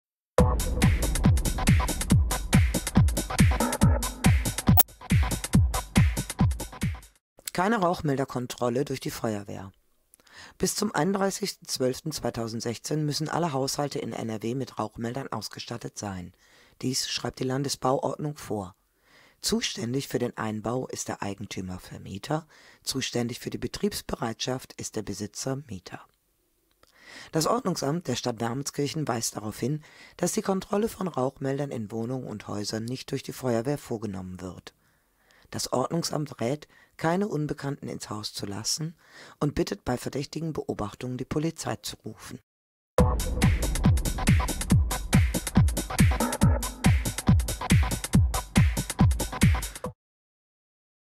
[Wk TV] Pressemitteilung: Rauchmelder
Bis wann muss sich wer um den Einbau von Rauchmeldern in Wohnungen kümmern - und wer kontrolliert. Das Ordnungsamt der Stadt Wermelskirchen informiert kurz und gibt eine Warnung heraus.